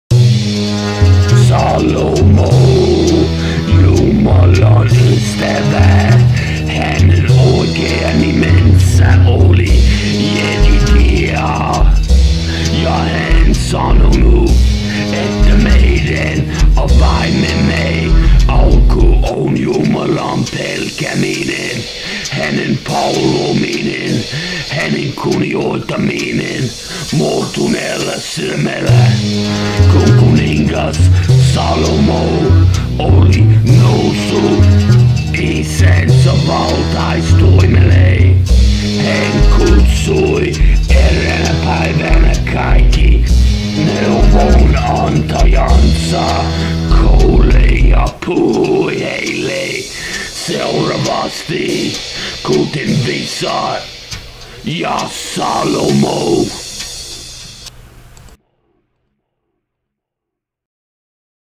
Salomo
Jumalan ystävä ... Hänen oikea nimensä oli Jedidiah ... ja hän sanonut, että meidän avaimemme alku on Jumalan pelkääminen, Hänen palvominen, Hänen kunnioittaminen murtuneella sydämellä ... Kun kuningas Salomo oli noussut isänsä valtaistuimelle, hän kutsui eräänä päivänä kaikki neuvonantajansa koolle ja puhui heille seuraavasti ... Kuten viisaat ja